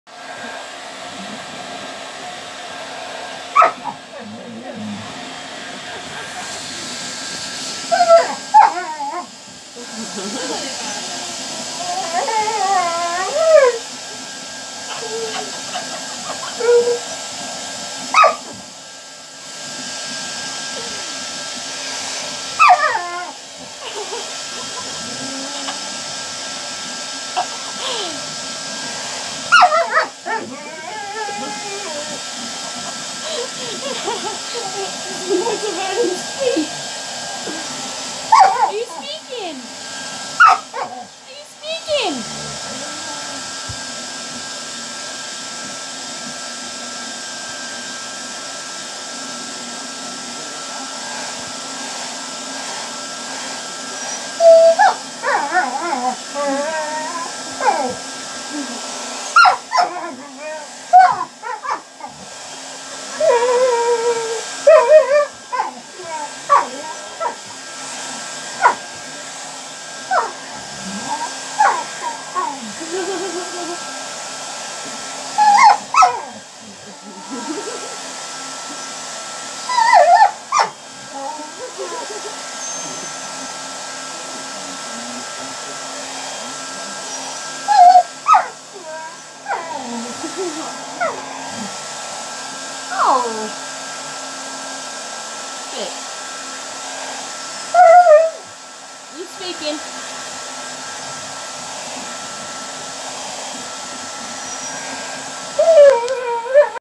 Puppy wants to eat hair dryer
Puppy going mad cos mum use her hair dryer to dry toes